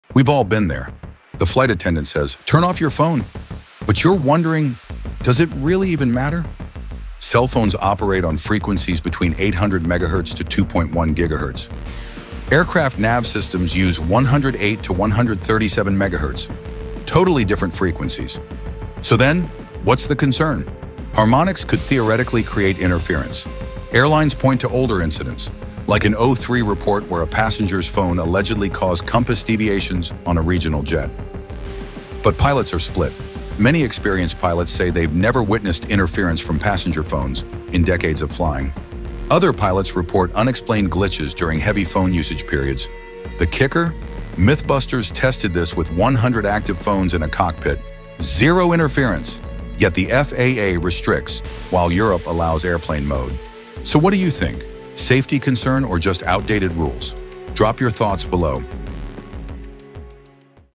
Airplane Mode: Does Your Phone Really Affect Flights?
DOES YOUR PHONE REALLY AFFECT FLIGHTS? ✈️

For years, airlines have told us to switch on Airplane Mode, but is it truly about safety, or just a precaution? In this short video, we uncover the science, the risks, and the truth behind one of aviation’s biggest passenger myths.